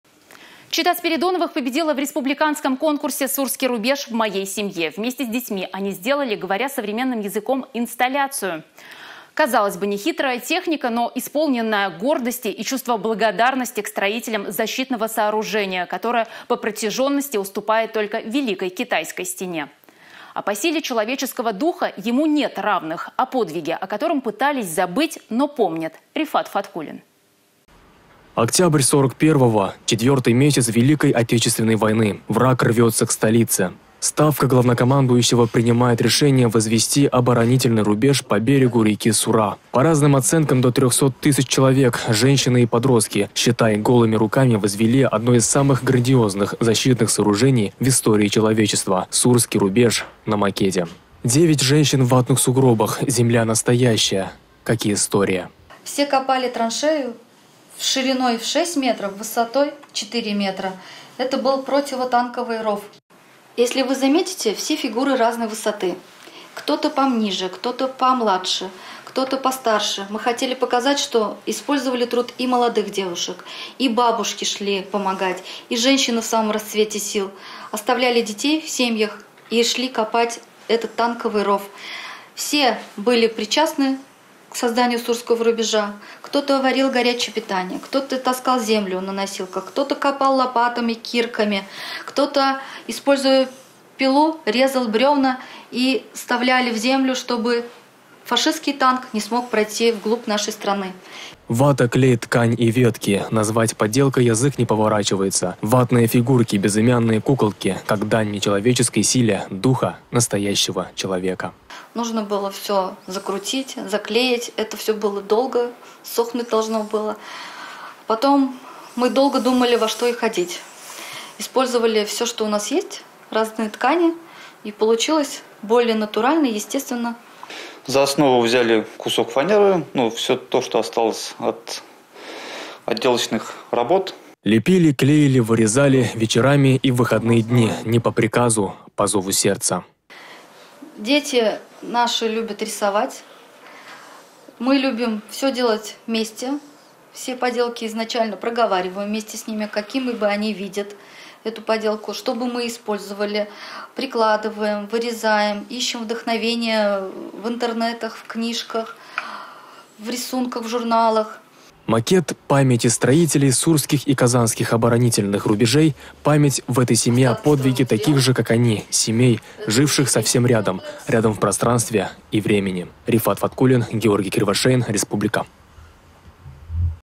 Реальная история в миниатюре
Чета Спиридоновых победила в республиканском конкурсе «Сурский рубеж в моей семье». Вместе с детьми они сделали, говоря современным языком, инсталляцию. Казалось бы, нехитрая техника, но исполненная гордости и чувства благодарности к строителям защитного сооружения, которое по протяженности уступает только Великой Китайской стене. А по силе человеческого духа ему нет равных. О подвиге, о котором пытались забыть, но помнят – Рифат Фаткуллин.